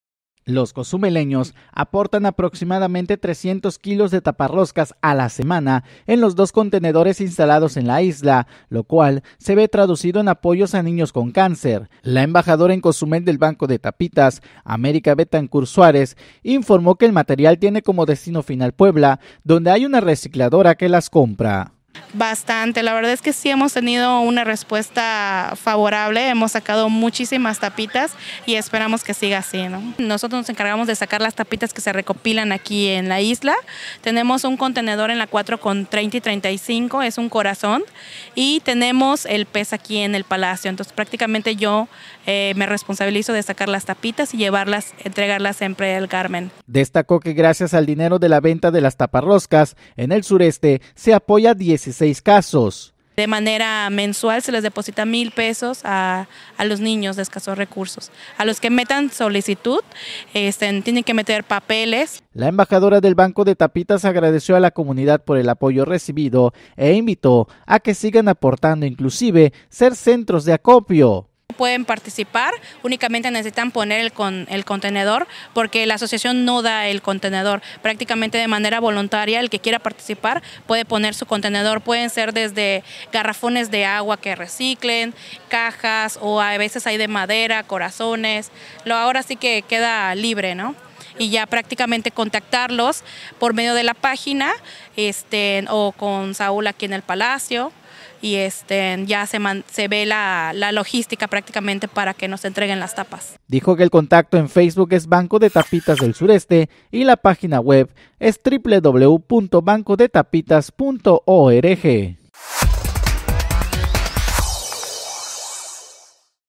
COZUMEL APORTA AL BANCO APROXIMADAMENTE 300 KILOS DE TAPARROSCAS A LA SEMANA
Los cozumeleños aportan aproximadamente 300 kilos de taparroscas a la semana en los dos contenedores instalados en la isla, lo cual se ve traducido en apoyo a niños con cáncer, la embajadora en Cozumel del banco de tapitas, América Betancur Suárez, informó que el material tiene como destino final Puebla donde hay una recicladora que las compra.